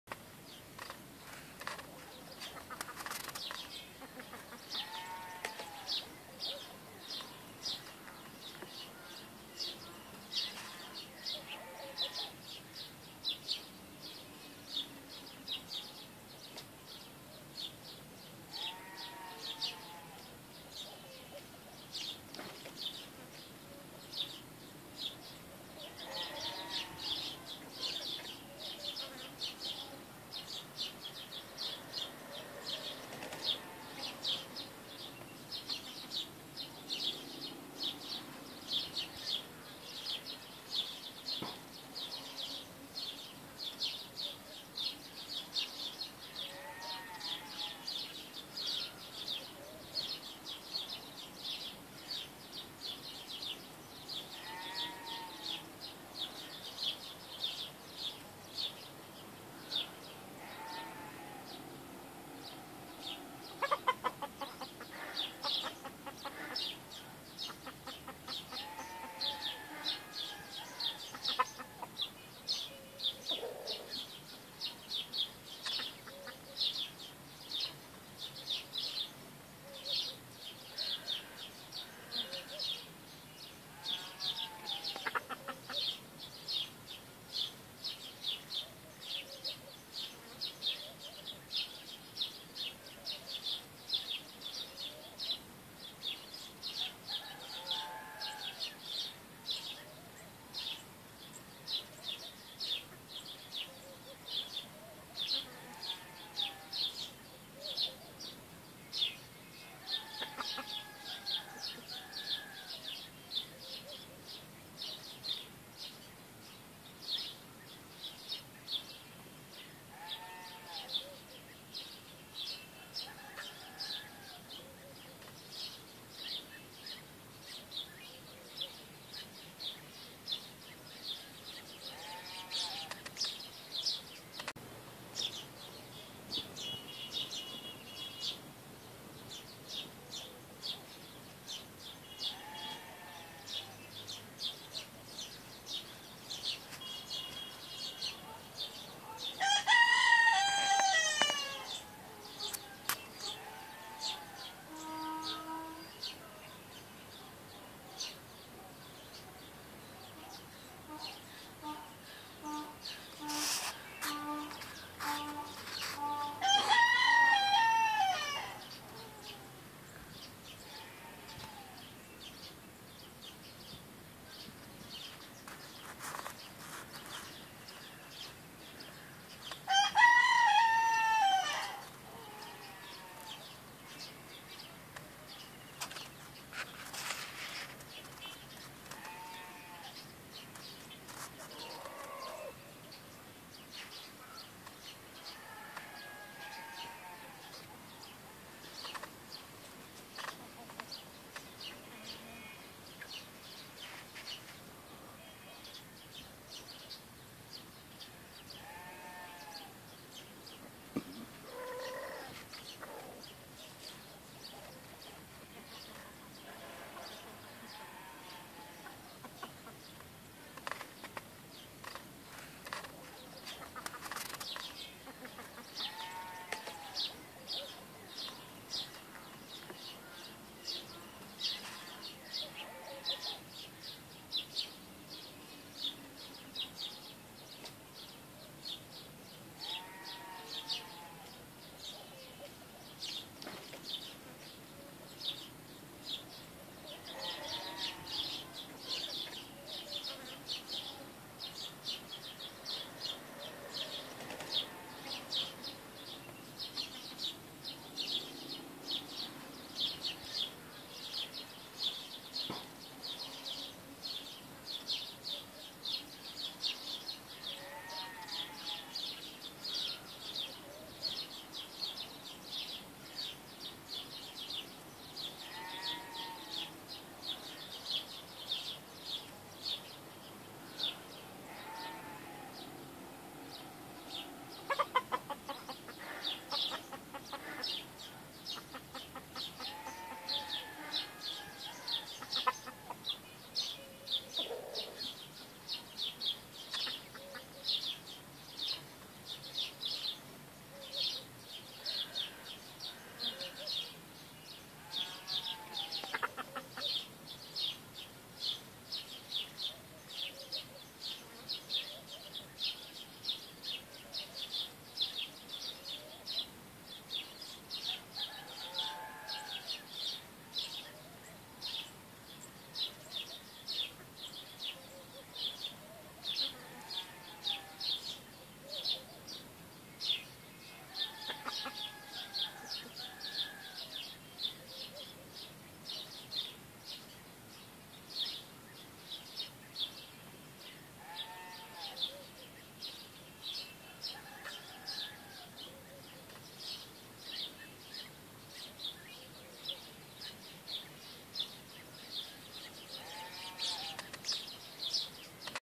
الدجاج البلدي فى الريف
ماذا ياكل الدجاج البلدي